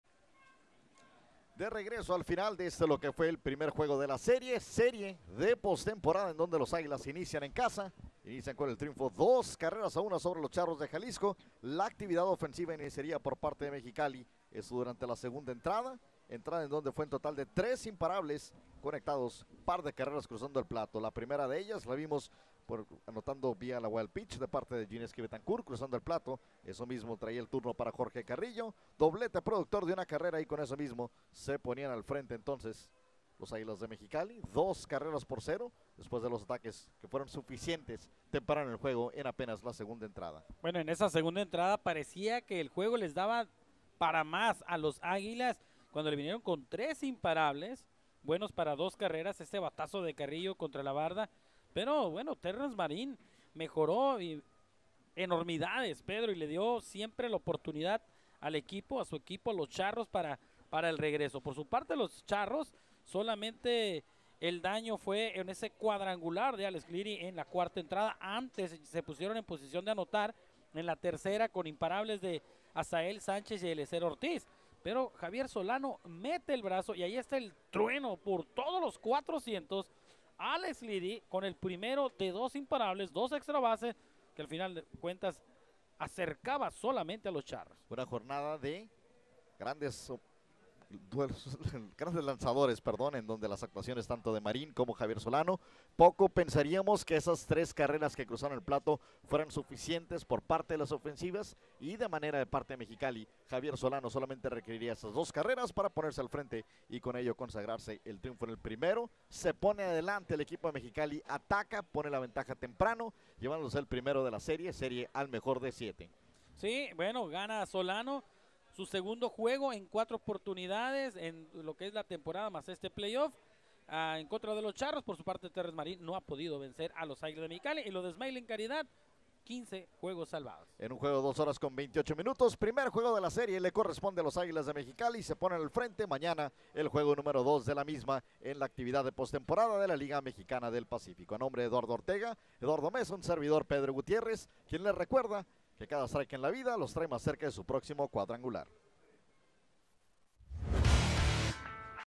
Resumen J1 PlayOffs2016 Charros de Jalisco Vs Aguilas de Mexicali 01/01/2016
Resumen del Juego 1 PlayOffs2016 Charros de Jalisco Vs Aguilas de Mexicali

Estadio BAir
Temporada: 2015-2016
Resultado: Charros 1 - Aguilas 2
Día de Juego: 1 de Enero de 2015